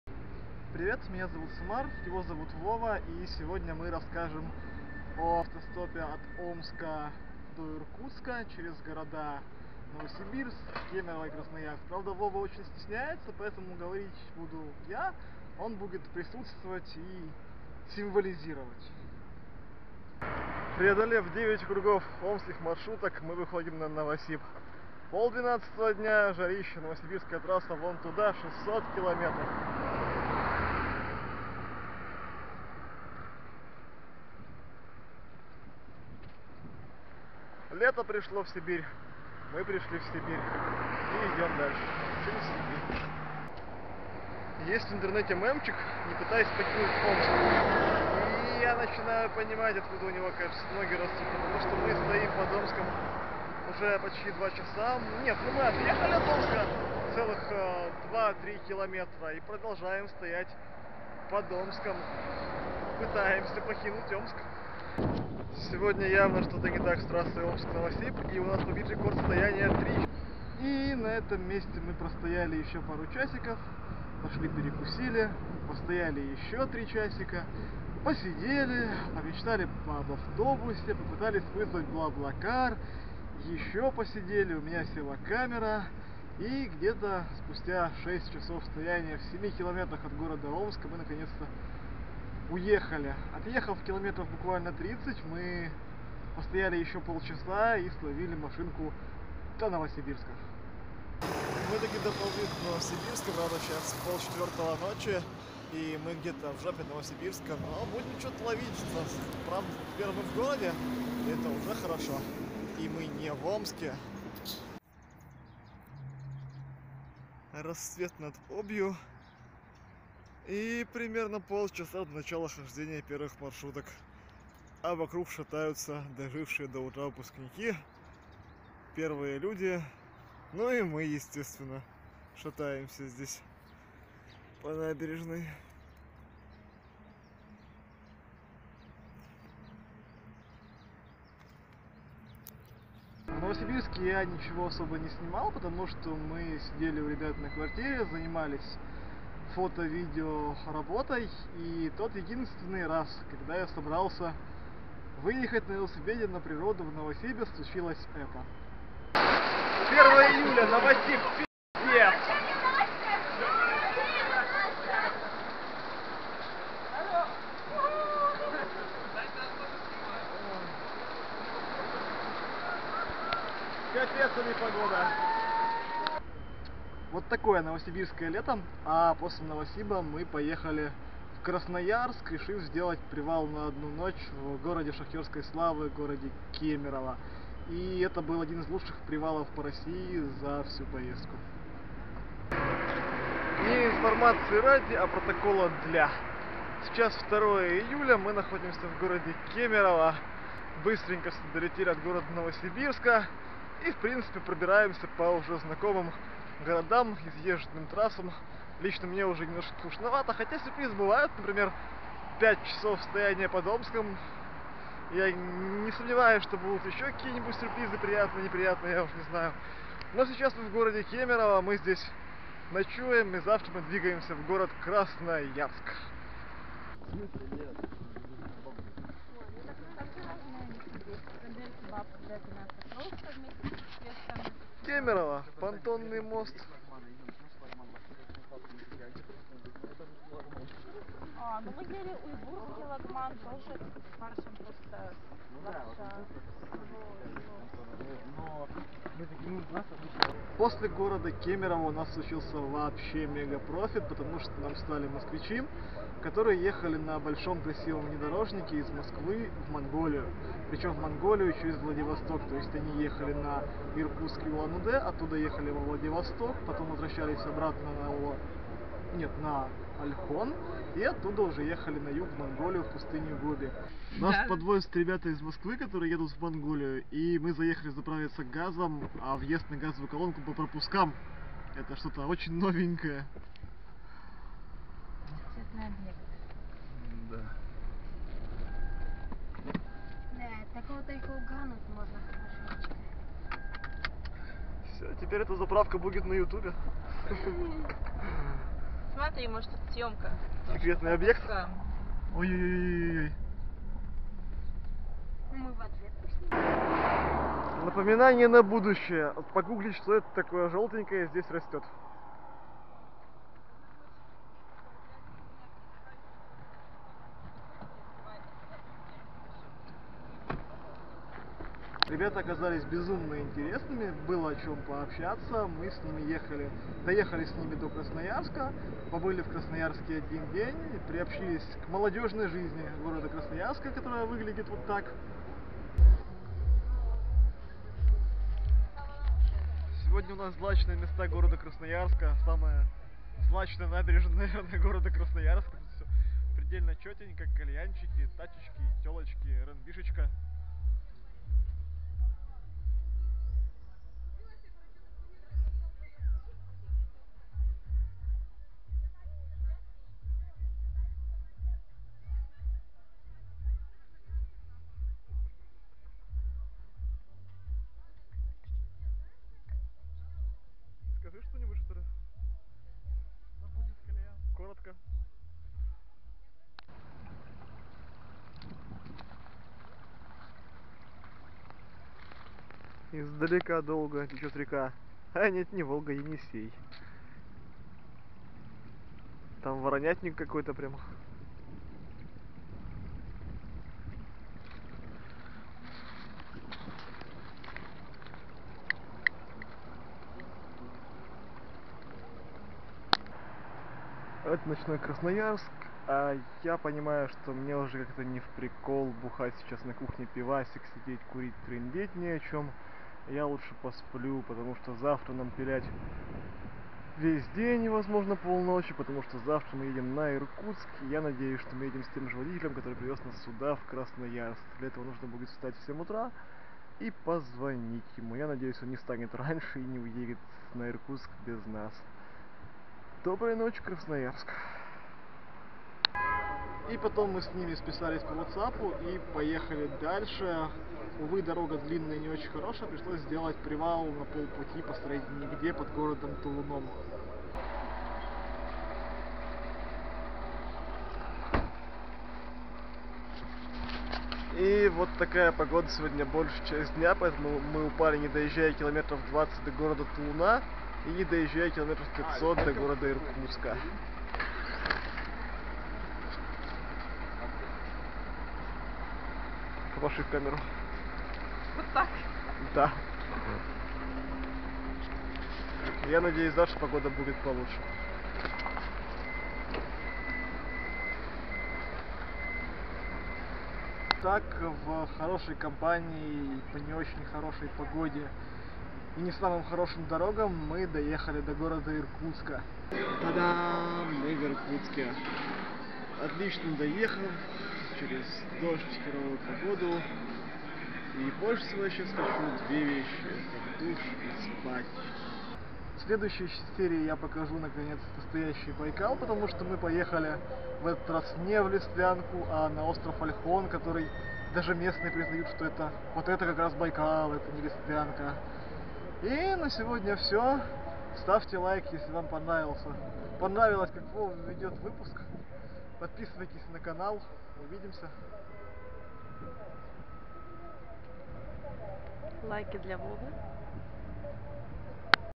Кругосветка. Часть 14: 3000 км вдоль транссибирской магистрали
Мы потихоньку углубляемся в Сибирь, преодолевая тысячекилометровые перегоны через леса.